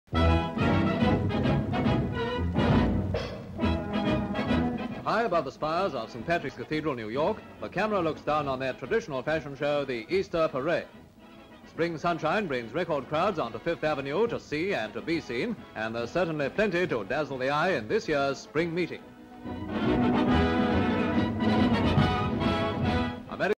Crowds gather in New York for Easter Parade (1947)
Fashions on display at St. Patrick's Cathedral Easter Celebration

Full Description:

SLATE INFORMATION: News highlights from across the Atlantic

USA: New York: New York City: St. Patrick's Cathedral:
EXT

NEW YORK Record crowds on Fifth Avenue for the Easter Parade

United States of America; Celebrations - Seasonal; New York

Background: Fashions on display at St. Patrick's Cathedral Easter Celebration